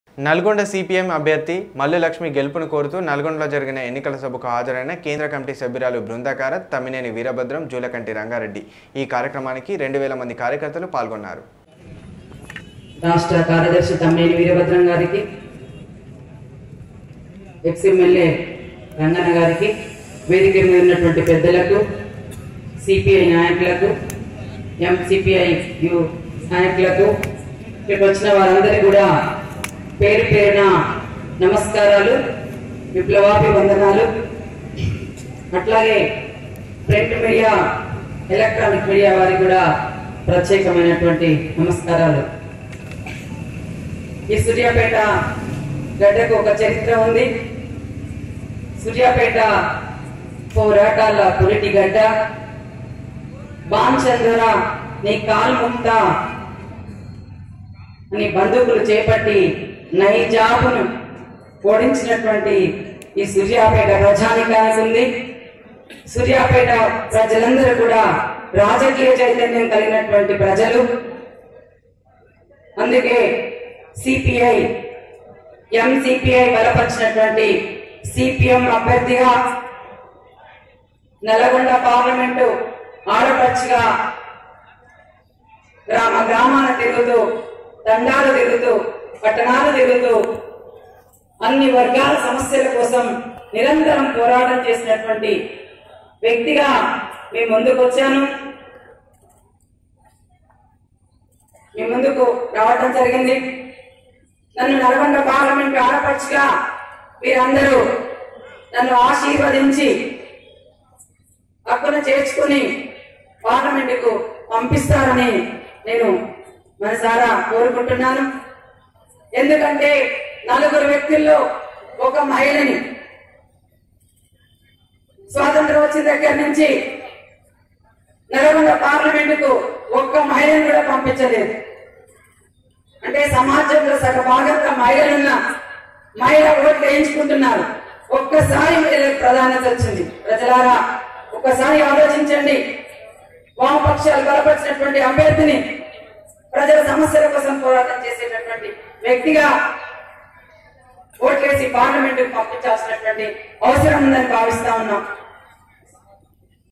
TS24 News నల్గొండ ఎంపీ సీపీఎం అభ్యర్థి మల్లు లక్ష్మీ గెలుపు కోరుతూ సభ
ఎంపీ సీపీఎం అభ్యర్థి మల్లు లక్ష్మీ గెలుపు కోరుతూ సభ